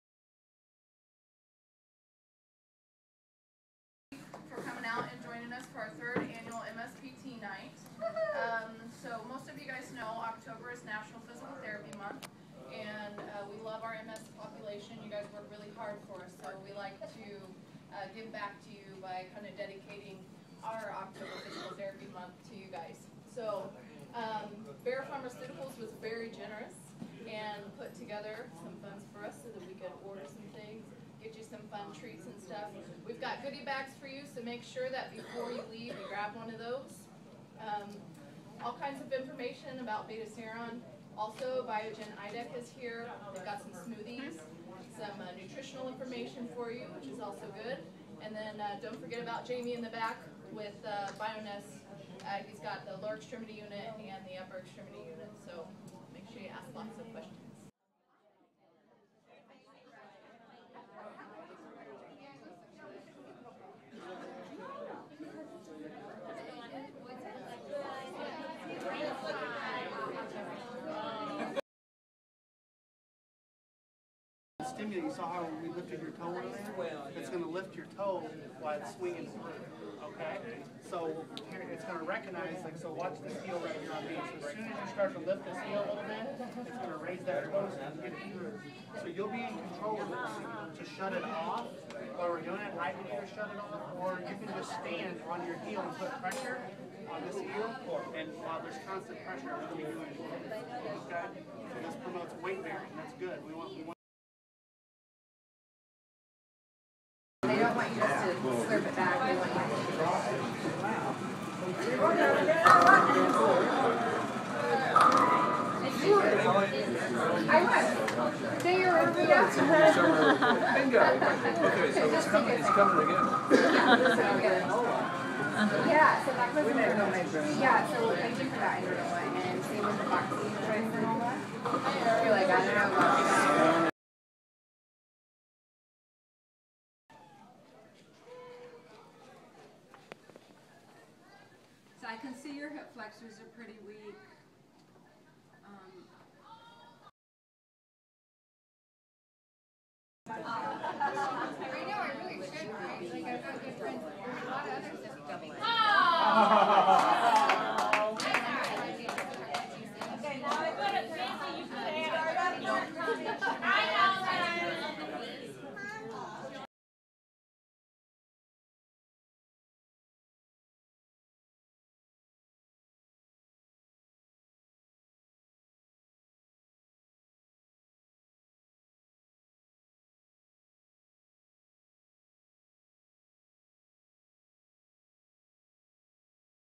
MS PT Night & Wii Bowling Tournament
October is designated as Physical Therapy Month and to celebrate, the physical therapists at Rowe Neurology Institute (RNI) wanted to give something back to a group of patients.  On October, 11th 2012, RNI hosted the 3rd Annual Multiple Sclerosis, Physical Therapy Night and Wii Bowling Tournament.

Turnout for this fun event was anticipated to be around 30 people so the event was moved from RNI's PT gym to an open room across the hall.  In all, 45 people came and participated in educational sessions, product demonstrations, food, fellowship and of course the Wii Bowling tournament on the big screen.